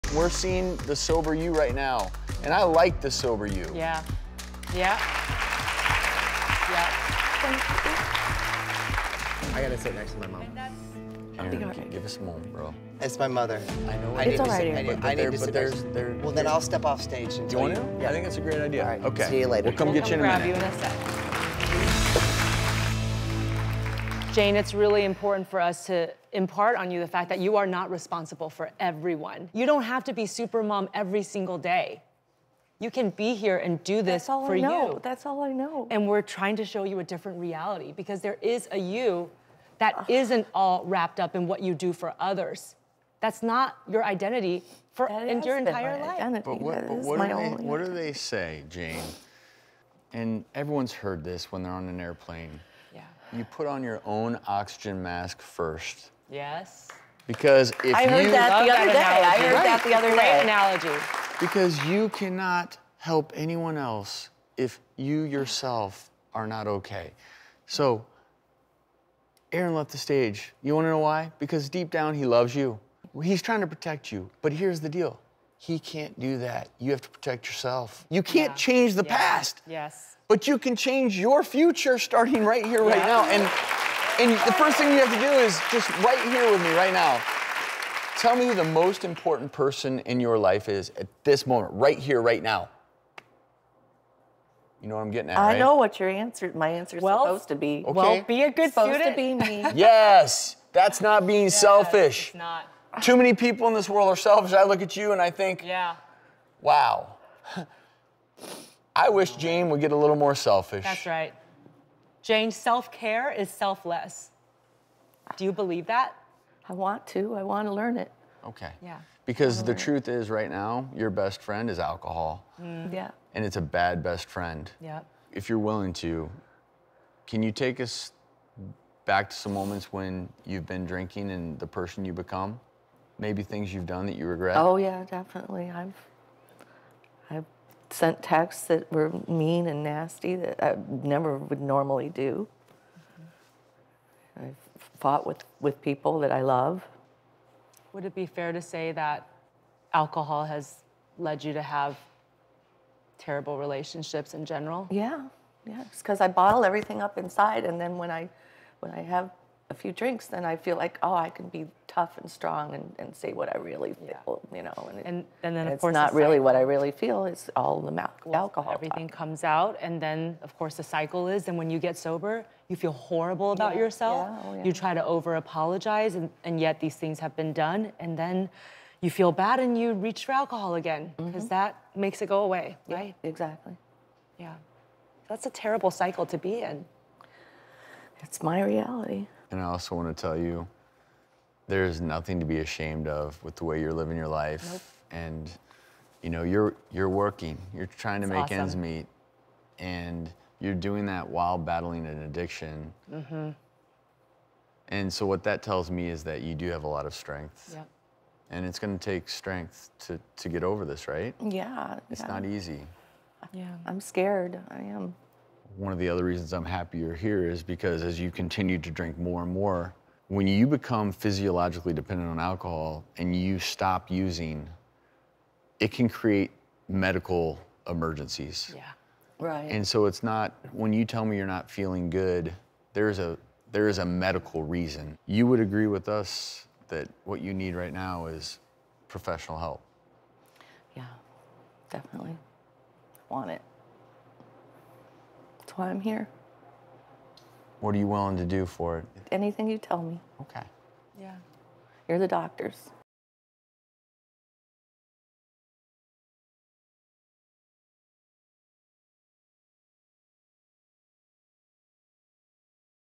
Can Popstar Mom Jane Carter Learn to Put Her Own Oxygen Mask on First?
Dr. Travis wants Jane Carter to know that you can’t change the past, but you can change the future. Dr. Travis Stork and Dr. Judy Ho encourage her to be a little selfish and to focus on self-care. They say that Jane’s best friend is alcohol, and it’s a bad best friend to have. She admits that she has sent mean and nasty texts, she has fought with people that she loves. She bottles everything up inside and when she drinks she can be tough and let it all out.